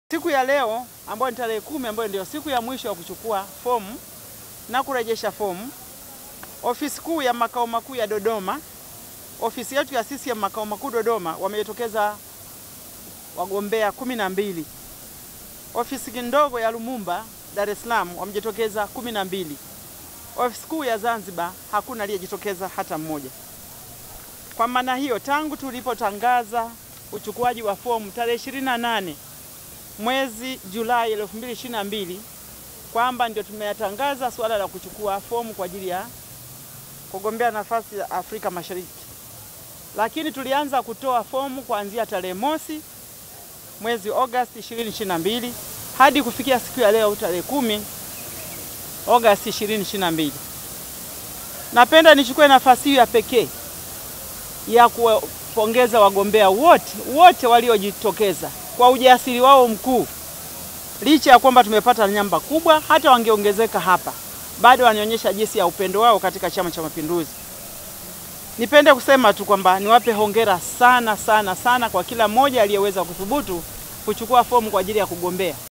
WAGOMBEA 178 WA UBUNGE WA BUNGE LA AFRIKA MASHARIKI (EALA) WAMECHUKUA NA KUREJESHA FOMU
Katibu wa Nec Idara ya Organaizesheni Dkt. Maudline Cyrus Castico kwa niaba ya Katibu Mkuu wa CCM Ndg. Daniel Chongolo ametoa taarifa ya Mwenendo wa Uchukuaji na Urejeshaji wa Fomu za kuomba ridhaa ya Kuteuliwa kugombea Ubunge wa Afrika Mashariki (EALA) kupitia Chama Cha Mapinduzi.

Ikiwa taarifa ya kuchukua na kurejesha Fomu za kugombe nafasi za Ubunge wa Bunge la Afrika Mashariki (EALA) ilitolewa na Katibu wa Nec Idara ya Itikadi na Uenezi Ndg. Shaka Hamdu Shaka tarehe 28 July 2022 ambapo zoezi hilo la kuchukua na kurejesha Fomu lilianza Tarehe 01 Agosti 2022 na Kuhitimishwa leo Tarehe 10 Agosti 2022.

Akizungumza na waandishi wa Habari CCM Makao Makuu Jijini Dodoma Dkt. Castico amesema ikiwa leo ndiyo siku ya Kuhitimisha zoezi la Uchukuaji na Urejeshaji wa Fomu za kuomba ridhaa ya kuchaguliwa kuwa  Mbunge  wa Bunge la Afrika Mashariki amewapongeza wote waliojitokeza kuchukua na kurejesha Fomu.

“ Nichukue fursa hii kuwapongeza wagombea wote waliochukua na kurejesha fomu za kugombea Ubunge wa Bunge la Afrika Mashariki mmefanya uamuzi mzuri na wakishujaa wa kutimiza wajibu wenu wa kikatiba wa uanachama wa CCM wa kuchaguliwa au kuchagua,* Hongereni Sana”*Amesema Dkt. Castico

Dkt. Castico ametoa Rai kwa wagombea wote waliochukua fomu za EALA
“ Baada ya zoezi hili Kuhitimishwa leo naomba wagombea wawe watulivu, wawe na Subira na waendelee Kuchapa kazi ili kujenga Taifa hadi pale taarifa nyingine itakapotolewa na Chama”Amesema Dkt. Castico.

Baada ya kusema hayo Dkt. Castico amemalizia kwa kutoa taarifa ya Jumla kuu ya waliochukua na kurejesha Fomu za EALA tangu Tarehe 01 Agosti hadi leo  tarehe 10 Agosti 2022 katika vituo vitatu vya kuchukua na kurejesha Fomu hizo ni
Afisi kuu ya CCM Zanzibar, Ofisi ndogo ya CCM Lumumba na Makao Makuu ya CCM Jijini Dodoma Jumla ya *wagombea waliojitokeza ni 178 *

“Kwa idadi hii kubwa ya wagombea wa Ubunge wa Bunge la Afrika Mashariki walivyojitokeza kuchukua na kurejesha Fomu za kugombea ni Ishara kwamba CCM ndiyo Chama pekee kinachojali na kuzingatia Haki na Wajibu wa Wanachama wa CCM katika dhima ya Demokrasia” Amesem Dkt. Castico .